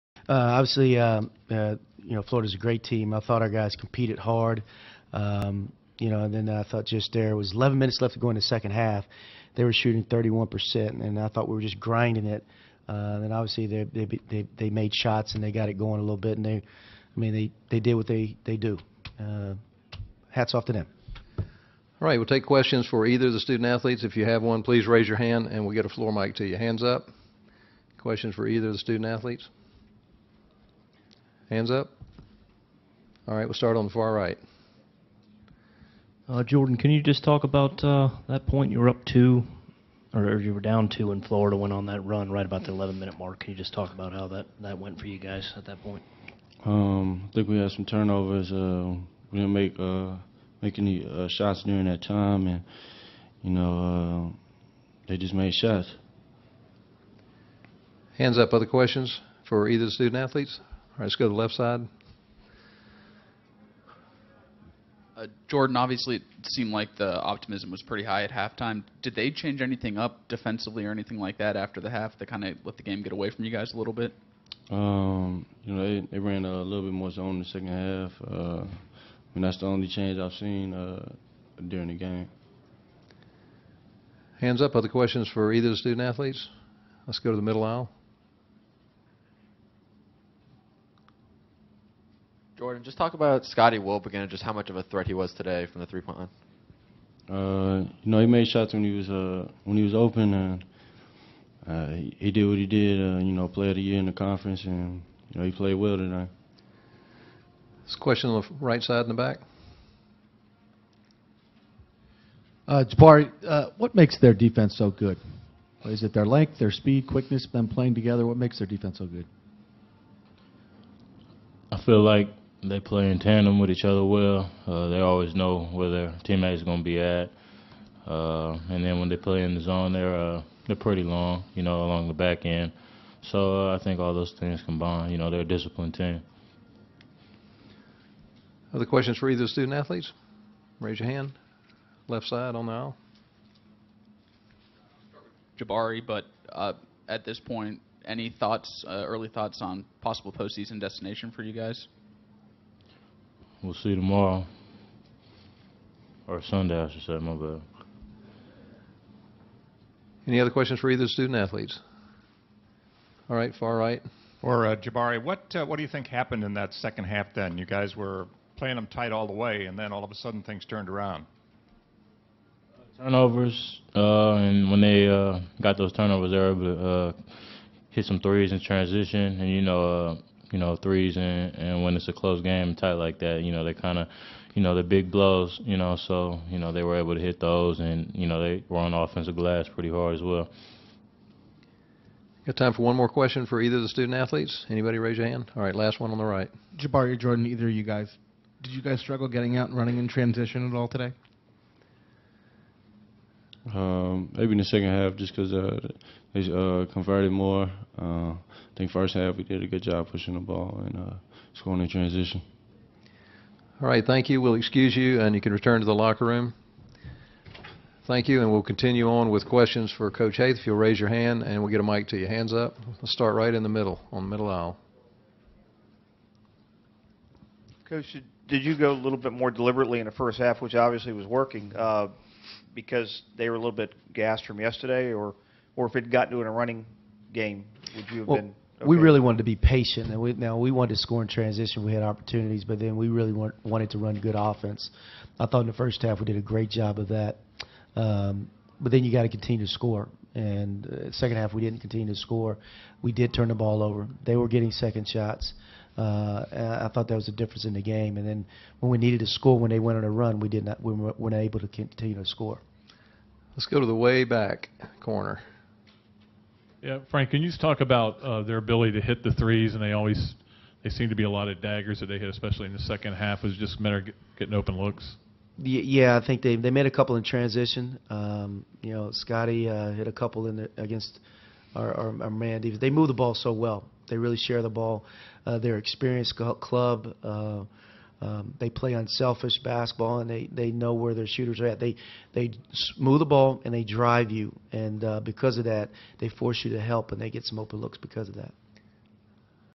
2014 SEC Men's Basketball Tournament - Quarterfinal Missouri Press Conference
Missouri is elimnated from the SEC tournament.